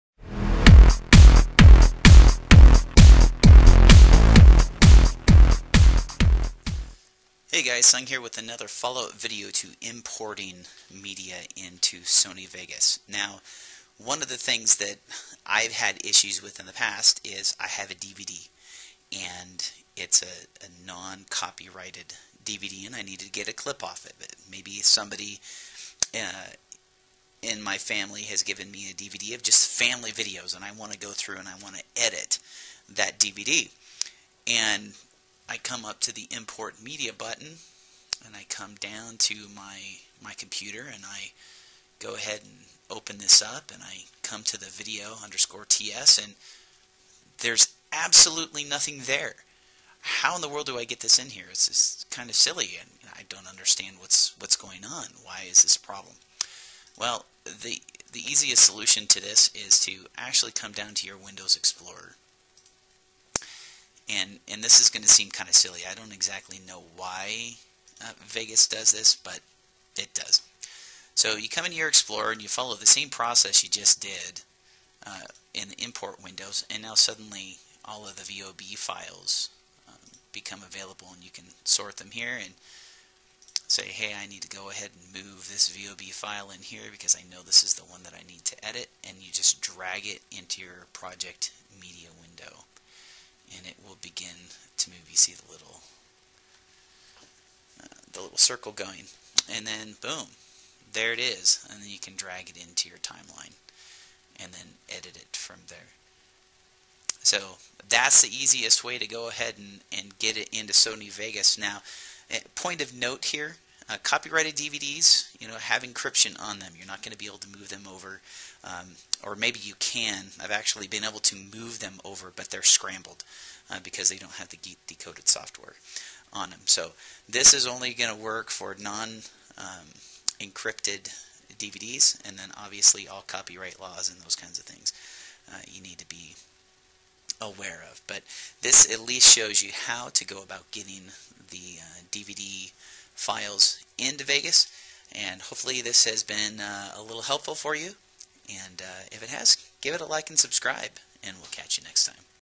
Sony Vegas Pro 12 Tutorial: Importing DVD files
In this tutorial we learn how to imported non copyrighted DVD files into Sony Vegas for editing.